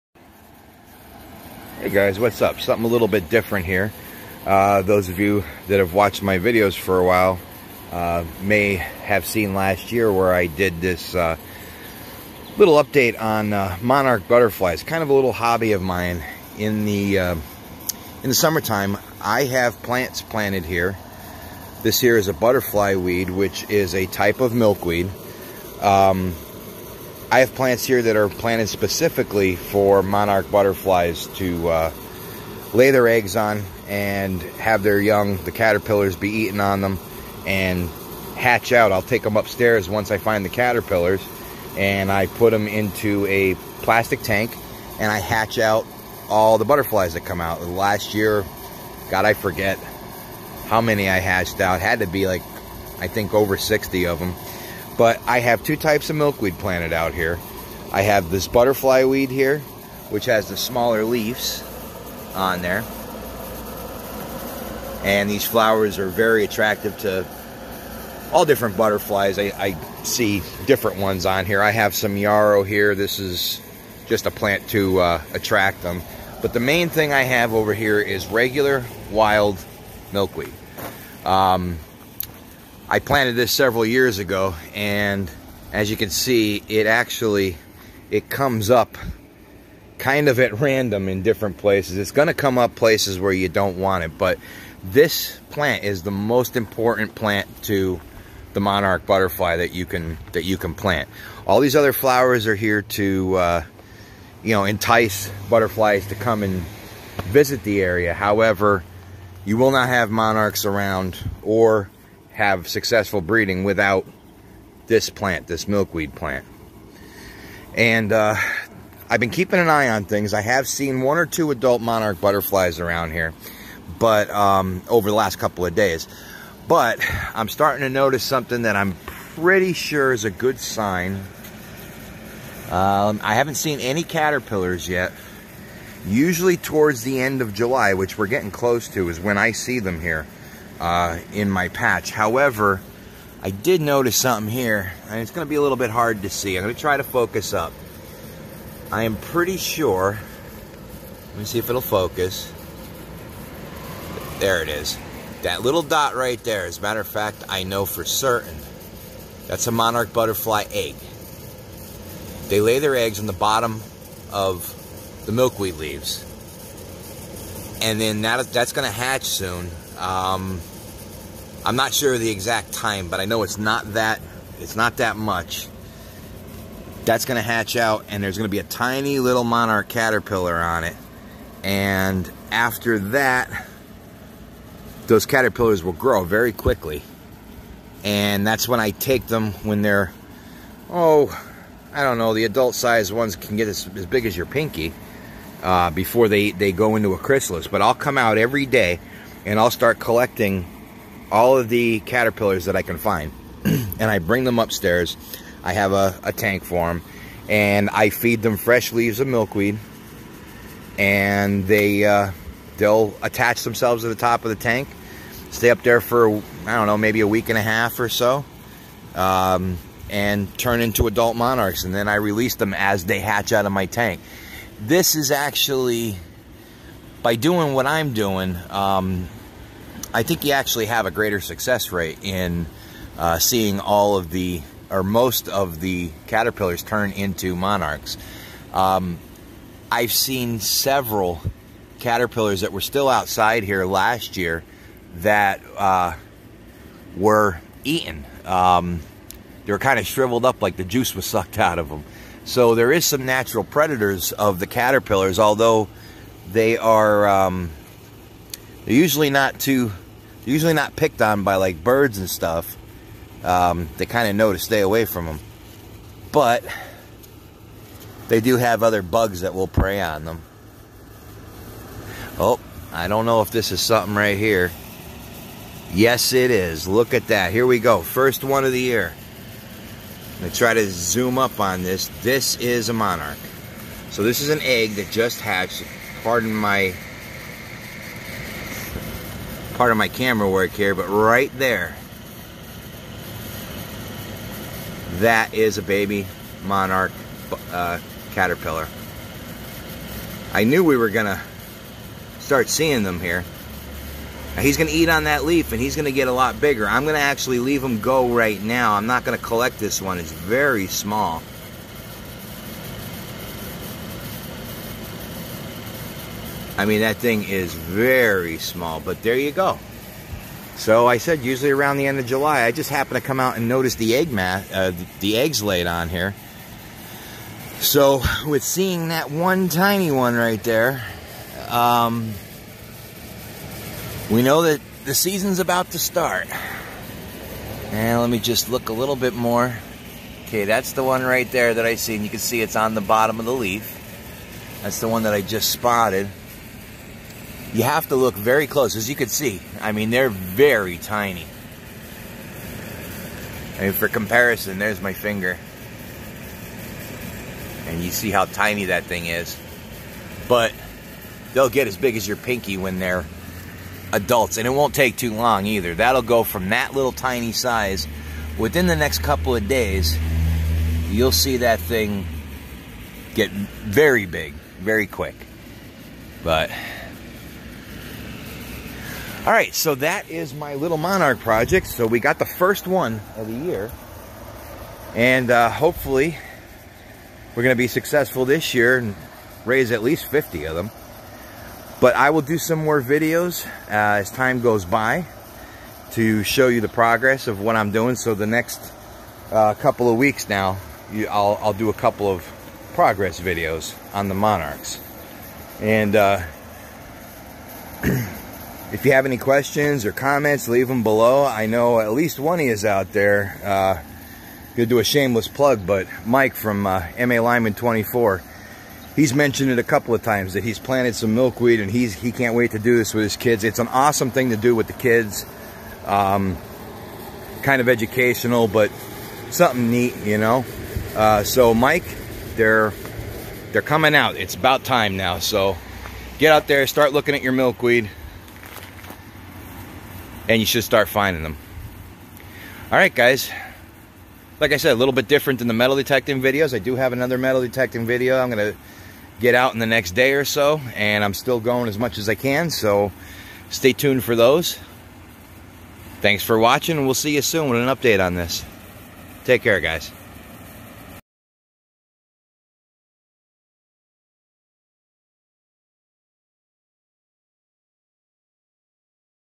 2019 Backyard Monarch Butterfly project
It's that time of the year again to begin one of my little hobbies, finding and raising Monarch butterflies from my own flower garden at home. Found the first signs of them today, come check out what I found on my milkweed! It's a bit different from my metal detecting videos, but I hope you will enjoy. New detecting video coming tonight or tomorrow!